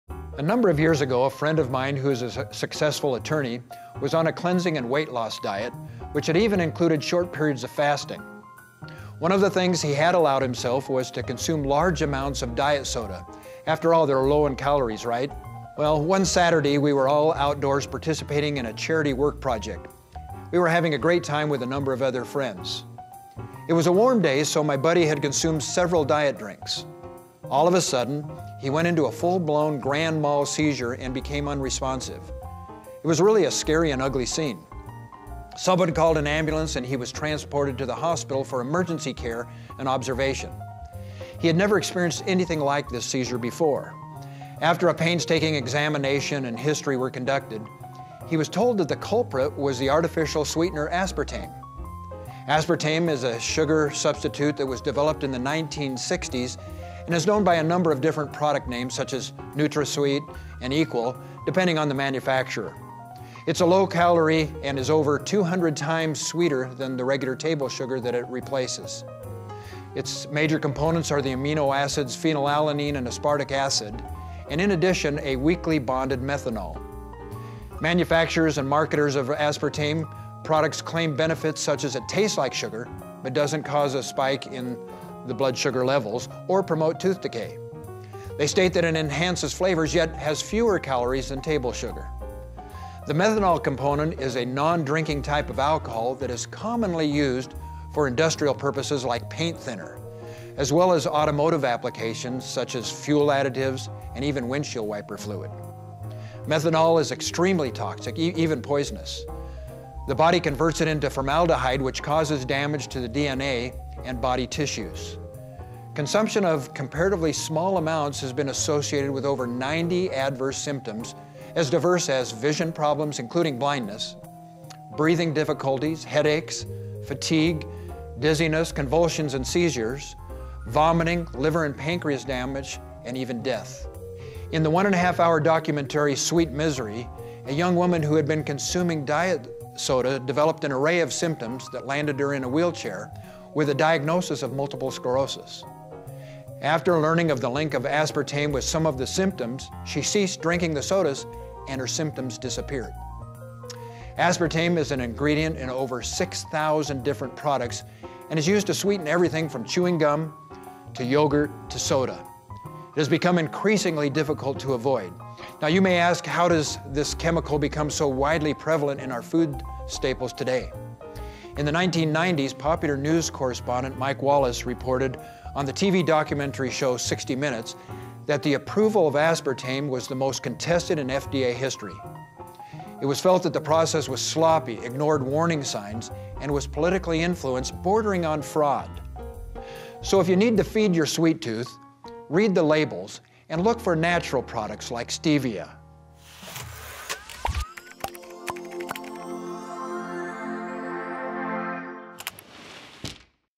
Aspartame Health Risks - A Silent Killer | Dr. Steve Weston
Aspartame tries to appear far more beneficial than it actually is, but it is far from the healthy alternative to sugar it would have you believe. Dr. Weston confronts aspartame this week. This low calorie sweetener may be much sweeter than sugar so it can be used to add sugary flavor without calories, but it hides a darker side. Aspartame is a combination of phenylalanine and aspartic acid with a weakly bonded methanol. Methanol is an alcohol that we should not drink. It is typically used in industrial and automotive applications, such as paint thinner, fuel additive, or windshield washer fluid. It is highly toxic, even poisonous. The body converts it into formaldehyde, which easily damages DNA and tissues. There are over 90 adverse side effects of methanol consumption, from seizures to blindness and even death. Over 6,000 products use aspartame. It is in yogurt, sodas, chewing gum, and much more, making it harder and harder to avoid. Natural sugars are a safer way to get your sweets. Stick to stevia, honey, maple, and coconut sugar.